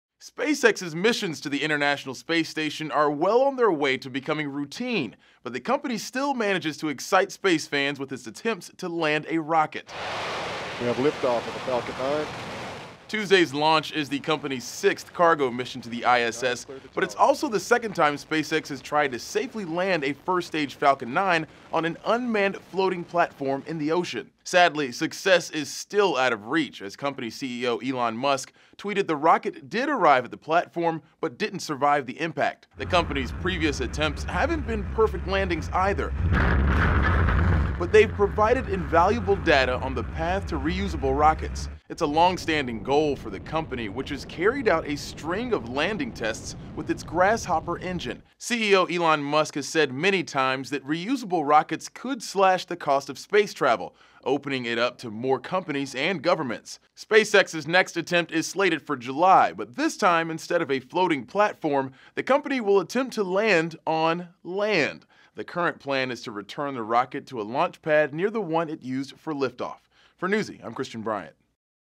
SpaceX's Rough Landing Isn't A Total Failure
SpaceX attempted to land another rocket after a space launch but didn't achieve its goal of a perfect landing.